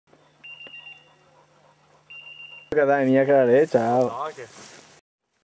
pernice con setter in ferma e 2di consenso
addestramento cani su pernici alta montagna